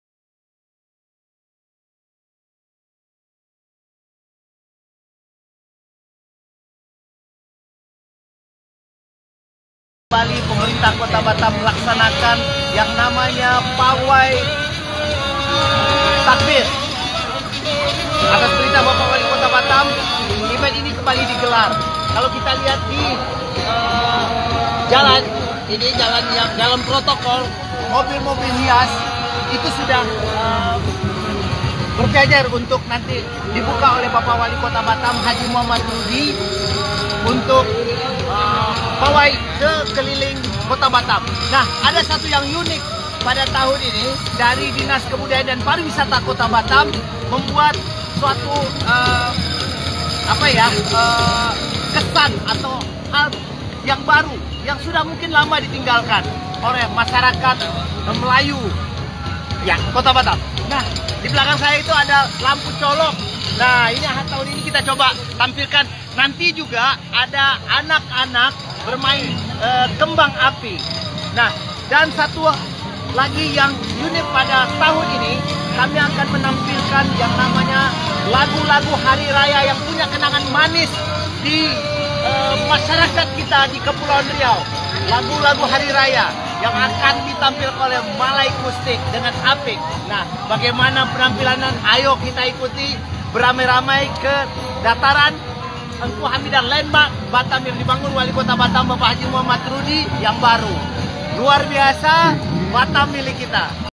MALAM TAKBIRAN KOTA BATAM 2022
Salah satu kegiatan yang diadakan kembali oleh pemerintah kota batam yaitu pawai malam takbiran kota batam 2022 yang mengambil rute dari dataran engku hamidah menuju ke simpang barelang.